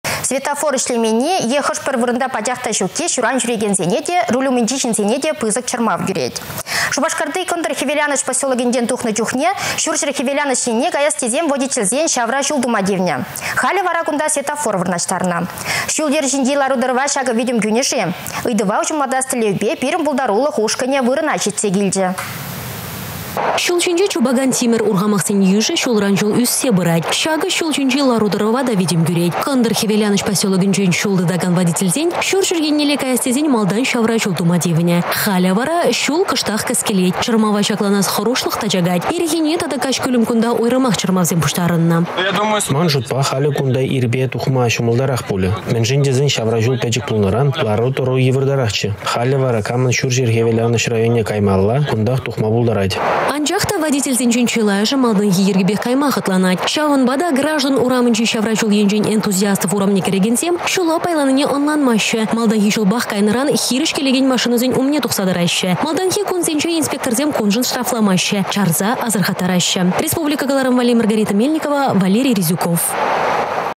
Çĕнĕ светофор вырнаçтарнă
Светофор ĕçлеменни е хăш-пĕр вырăнта  пачах та çукки  çуран çӳрекенсене те, руль умĕнчи çынсене те пысăк чăрмав кӳрет. Шупашкарти Кăнтăр-хĕвеланăç поселокĕнчен тухнă чухне çурçĕр-хĕвеланăç енне каяс тесен водительсен çавра çул тума тивнĕ. Халĕ вара кунта светофор вырнаçтарнă. Çул-йĕр çинчи лару-тарăва çакă витĕм кÿне-ши? Ыйтăва уçăмлатас тĕллевпе пирĕн пултарулăх ушкăнĕ вырăна çитсе килчĕ.